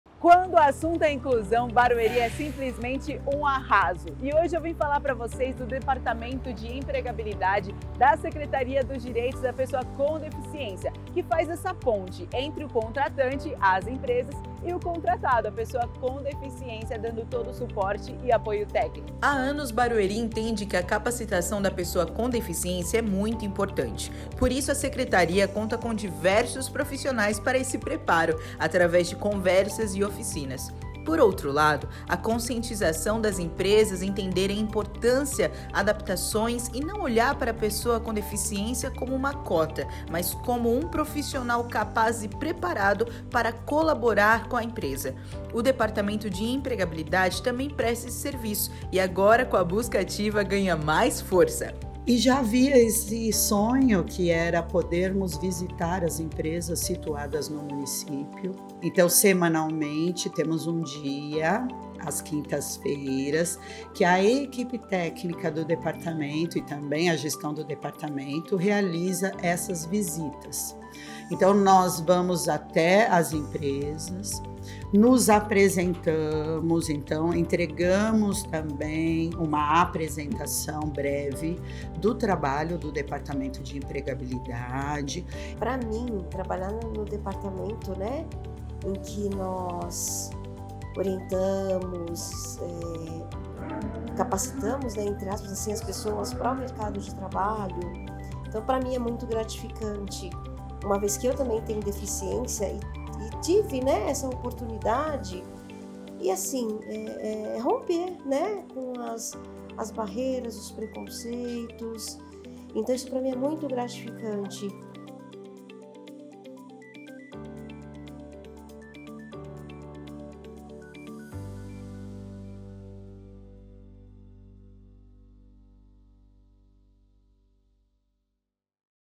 Empregabilidade na SDPD
A Secretaria dos Direitos da Pessoa com Deficiência se empenha cada dia mais para ajudar a transpor barreiras que a pessoa com deficiência encontra para o ingresso no mercado de trabalho, dando apoio para o contratado, sua família e à empresa contratante.

Aperte o play e confira a matéria sobre a empregabilidade na SDPD